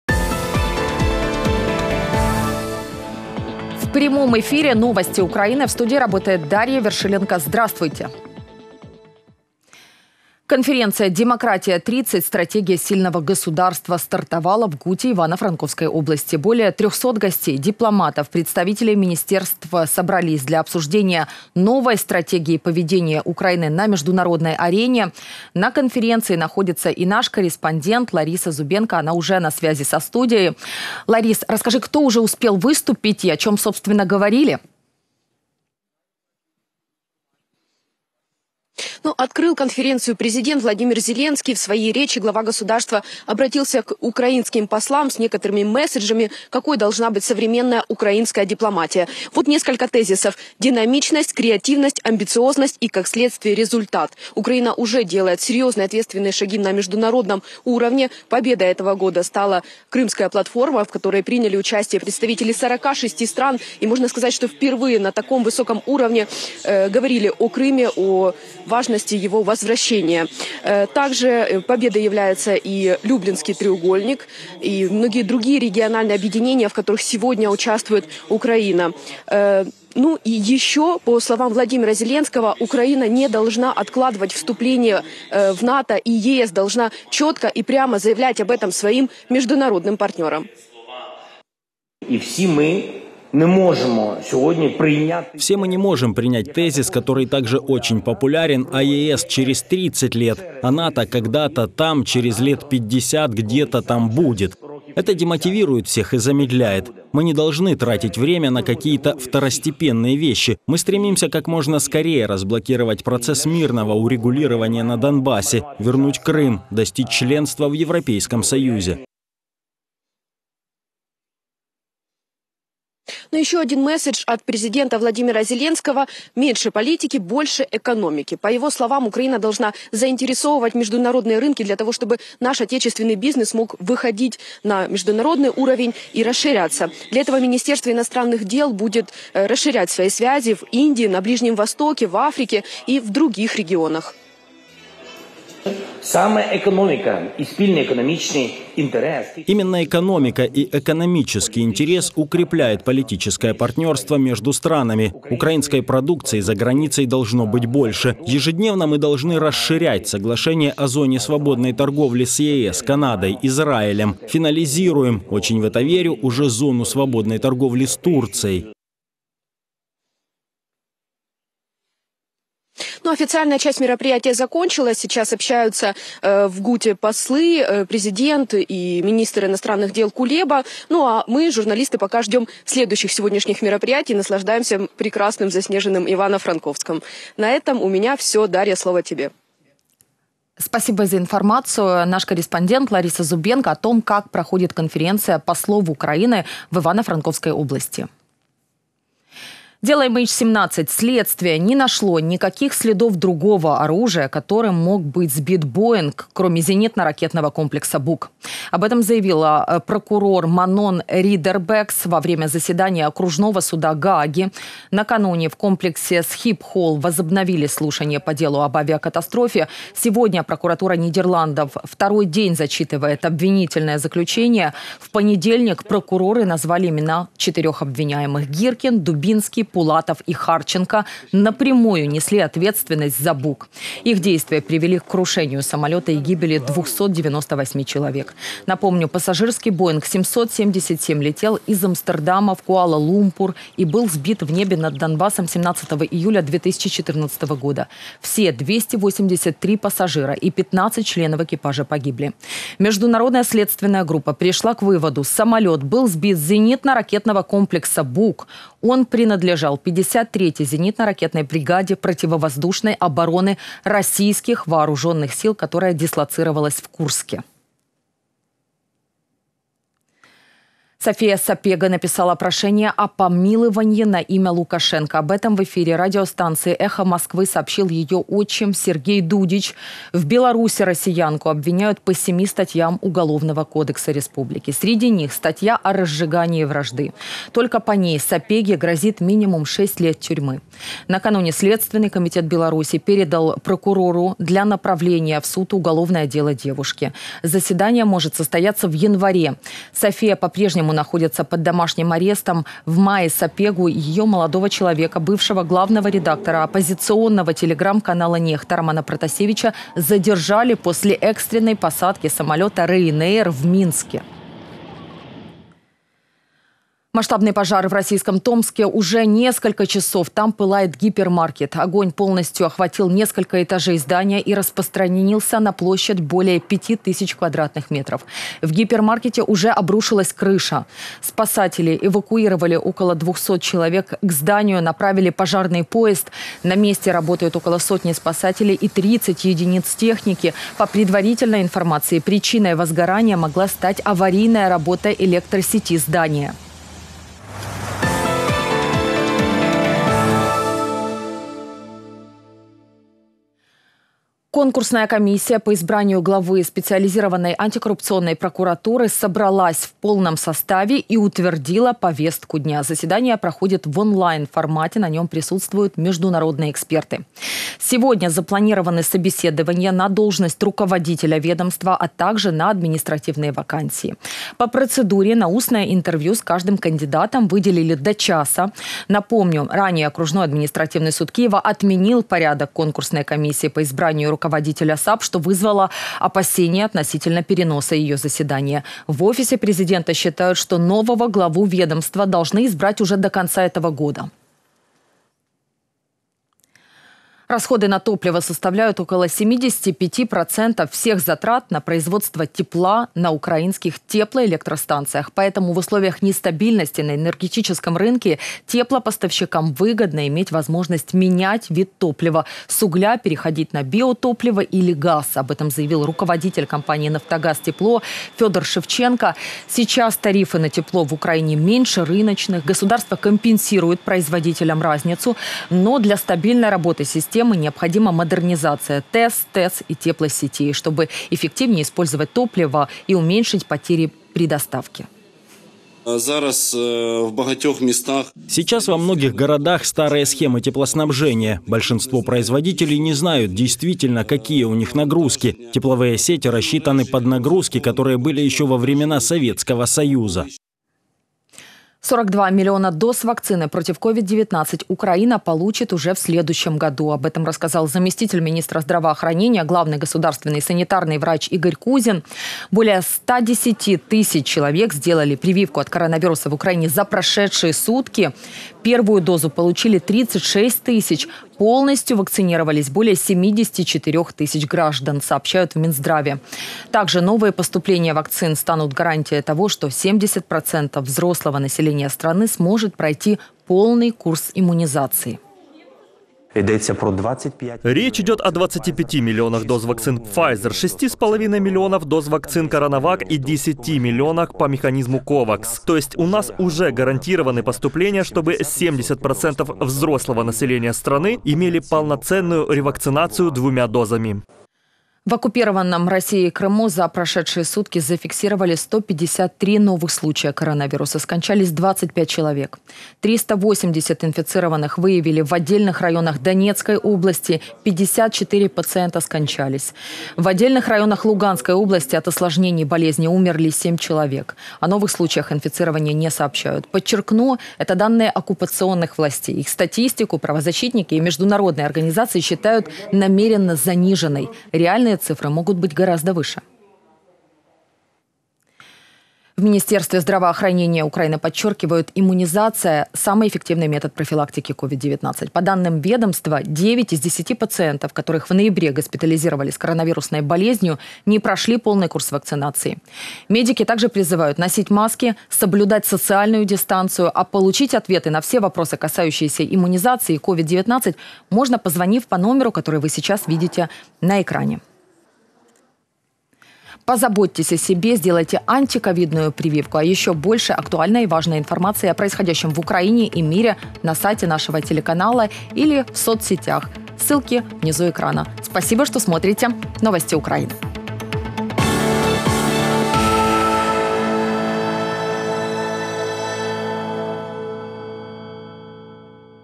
Стратегия дипломатии Украины. Главные тезисы | День 21.12.21
00:00 Смотрите в этом выпуске:
00:10 Конференция "Демократия 30. Стратегия сильного государства" стартовала в Гуте Ивано-Франковской области.
03:19 Дело MH17. Следствие не нашло никаких следов другого оружия, которым мог быть сбит "Боинг", кроме ЗРК "Бук".
04:30 София Сапега написала прошение о помиловании на имя Лукашенко.
05:16 Масштабный пожар в российском Томске. Уже несколько часов там пылает гипермаркет.
05:54 Конкурсная комиссия по избранию главы САП собралась в полном составе и утвердила повестку дня.
06:36 Расходы на топливо составляют около 75% всех затрат на производство тепла на украинских теплоэлектростанциях.
07:31 42 млн доз вакцины против COVID-19 Украина получит уже в следующем году.
08:25 В оккупированном Россией Крыму за прошедшие сутки зафиксировали 153 новых случая коронавируса.